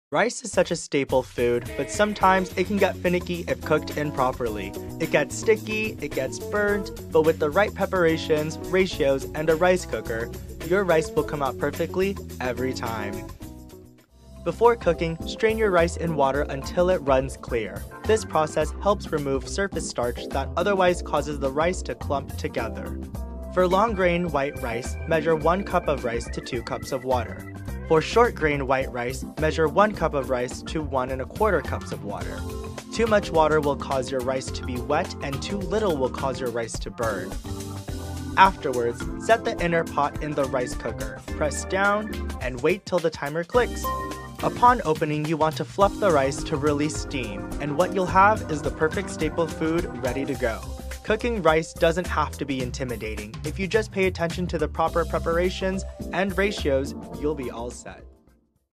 How to Cook Rice (Vyond)
Hi! As part of an interview process, I had to create a 40-60sec how to animation using Vyond (a cloud based app – think wixsite, but for animation) Enjoy!

I enjoyed the every process of creating this video! Woop woop.